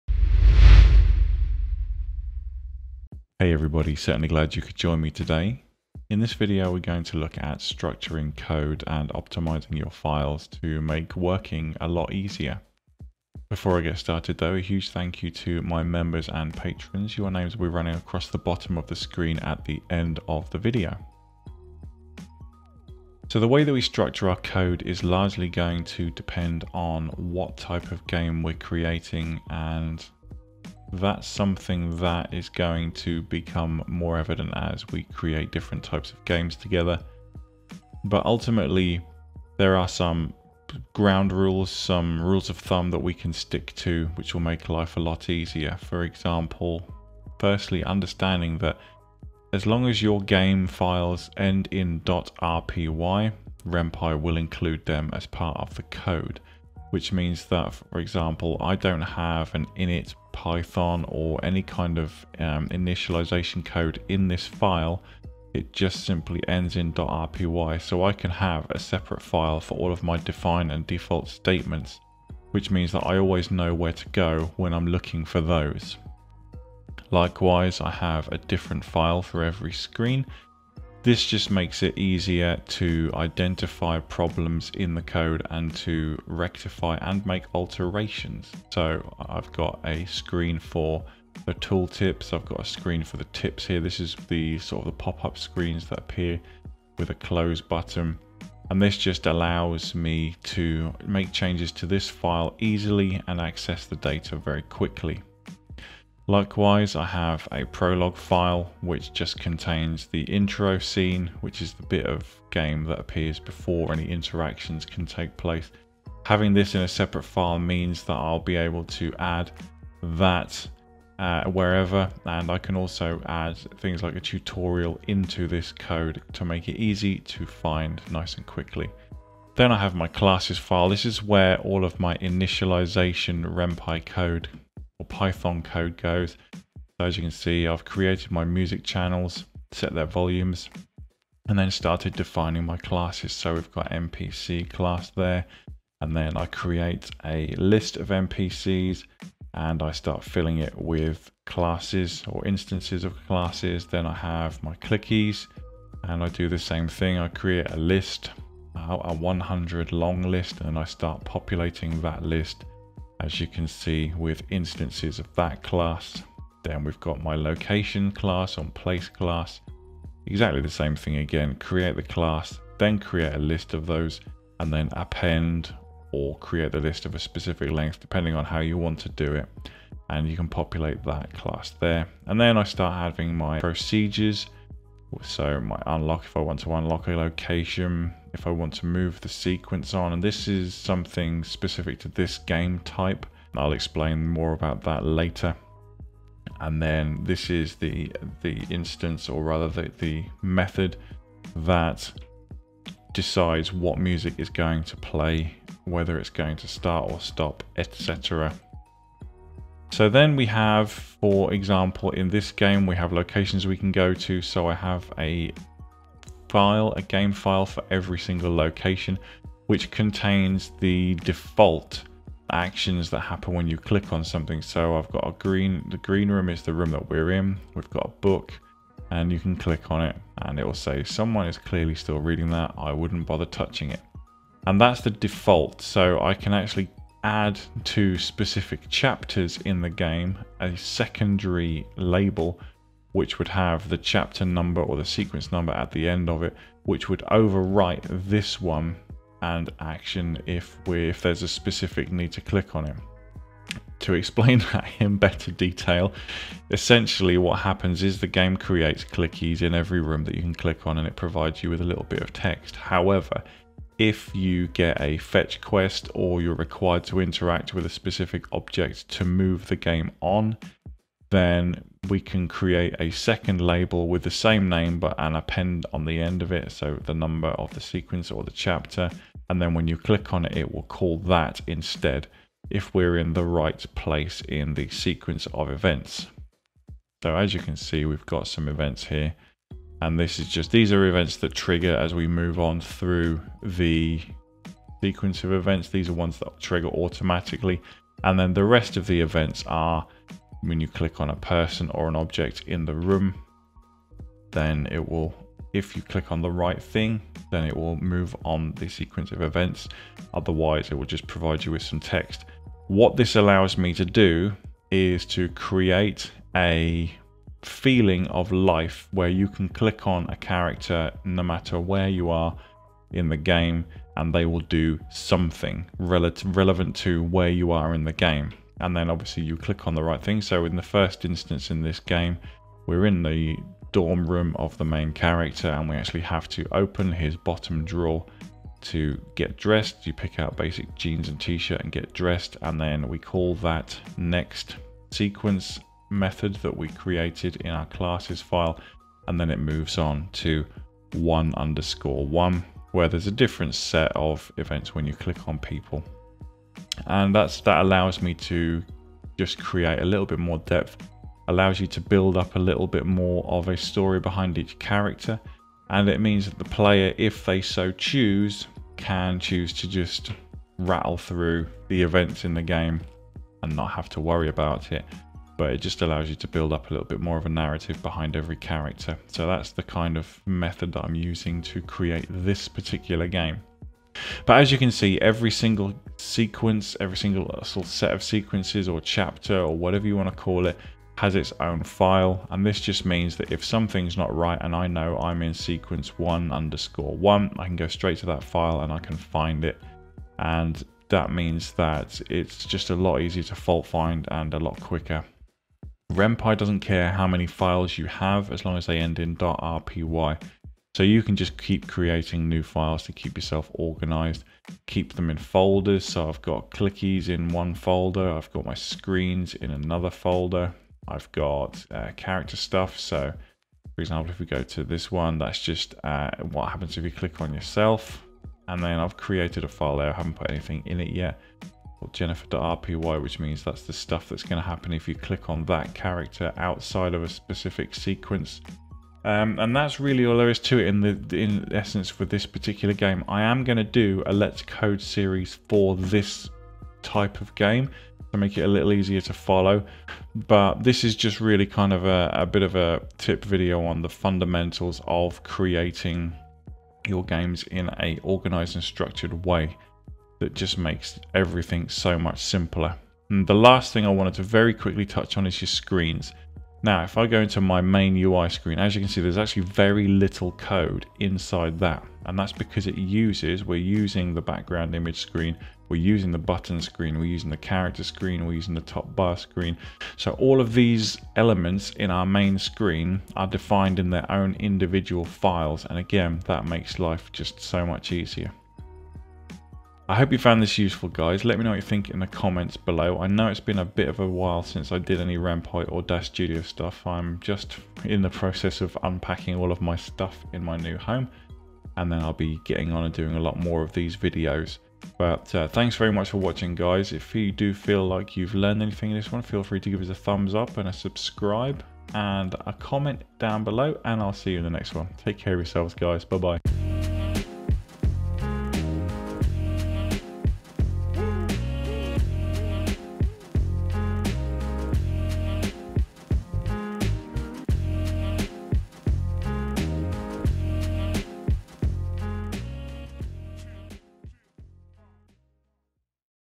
[Ren'Py] Organising your code
In this video we look at how I lay out my files and code to improve my organization and workflow

All information contained in this video is correct to the best of my knowledge at the time of recording.

Links